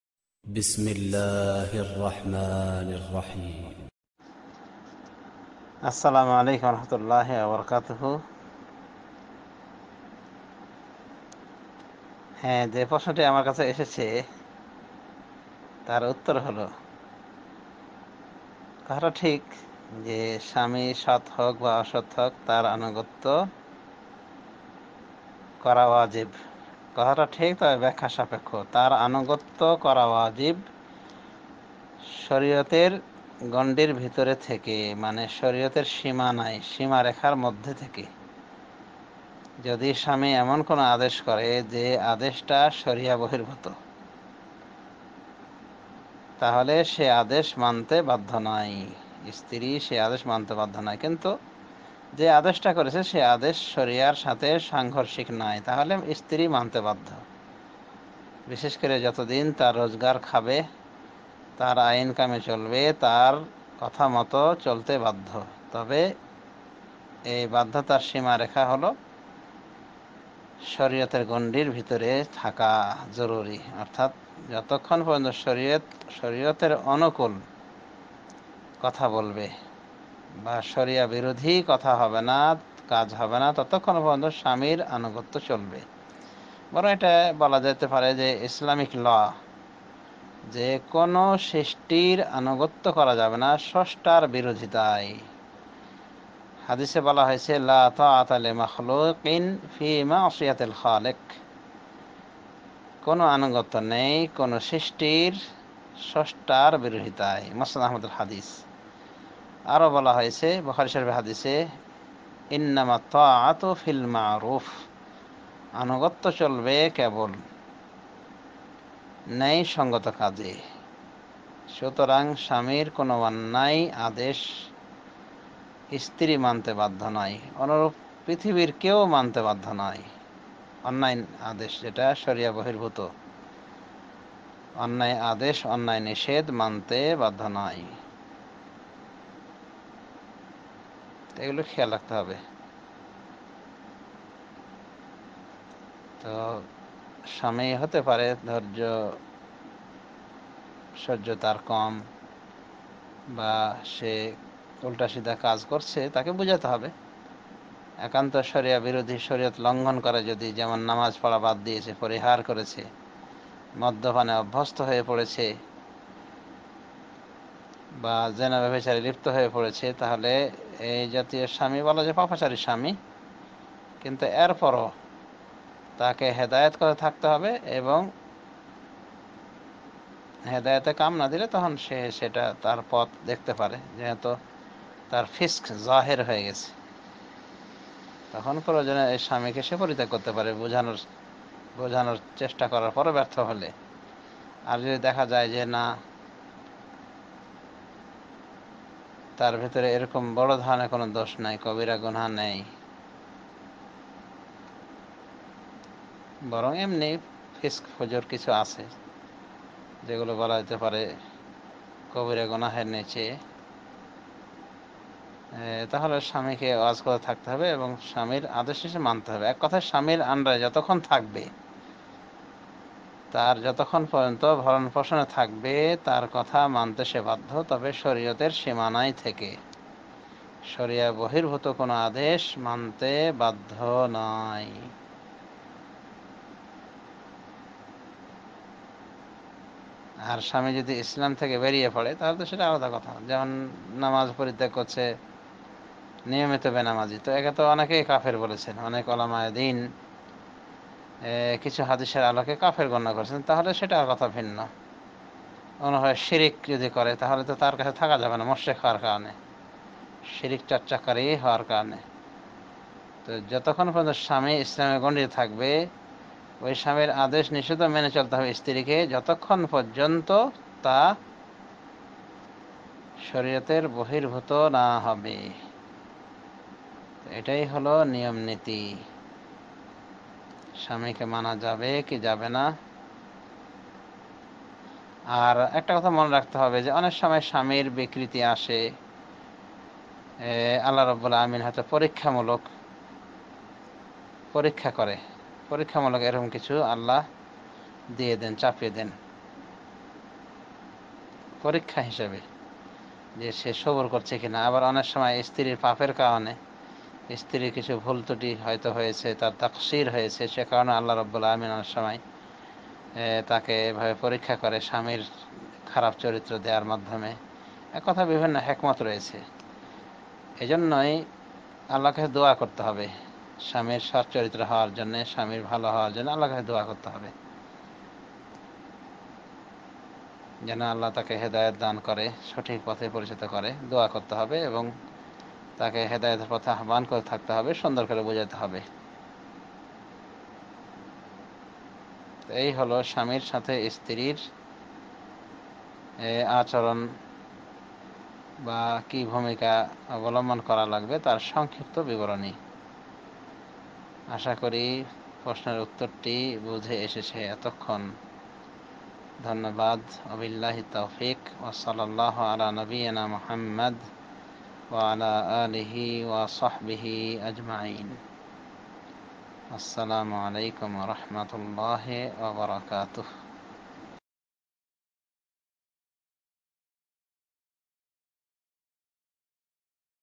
Sorbo Obosthai Ki Samir Anugotto Kora Wajib? Sheikh Akhtarul Aman Madani|waz|Bangla waz
Sorbo Obosthai Ki Samir Anugotto Kora Wajib?